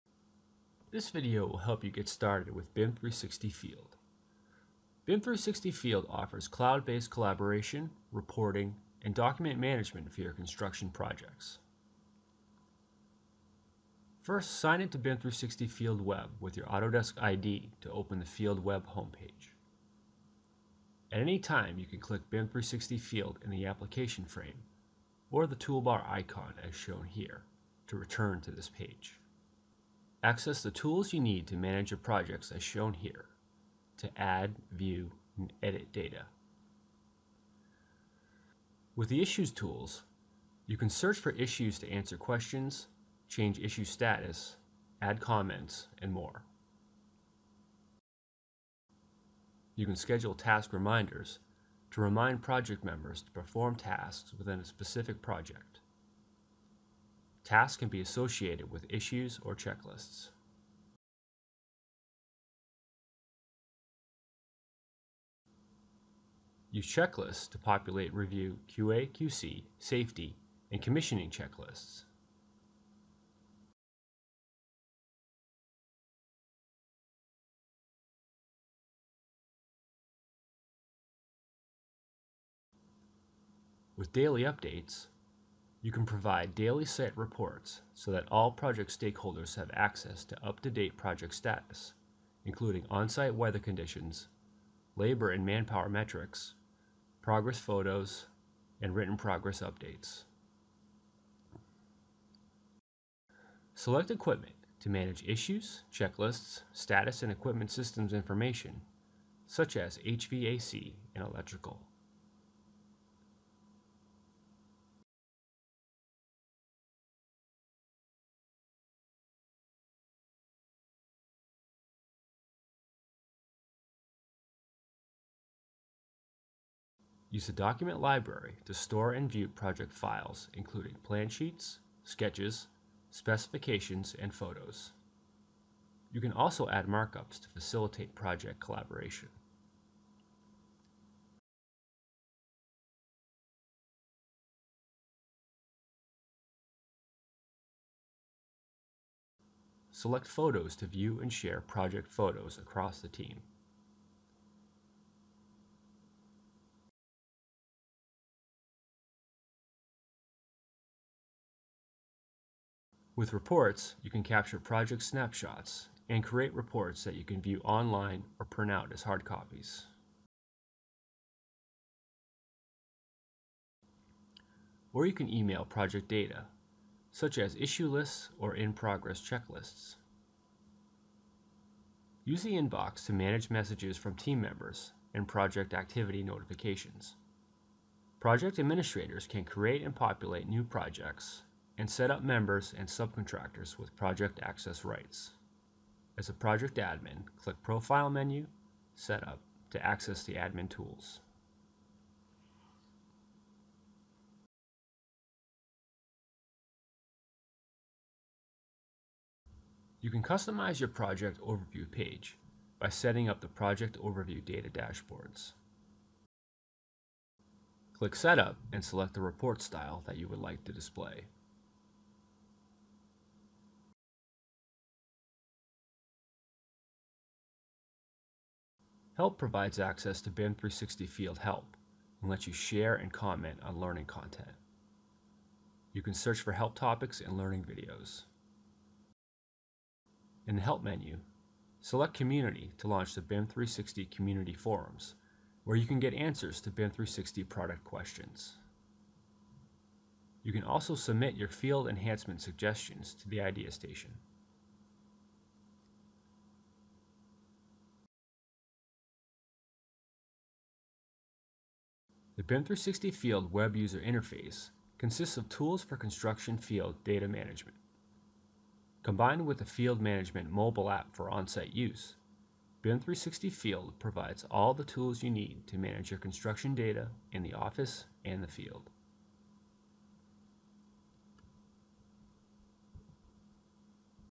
BIM 360 Field Overview - February 2015
Take a tour of the BIM 360 Field user interface and learn about the tools you need for field data management, commissioning, and handover.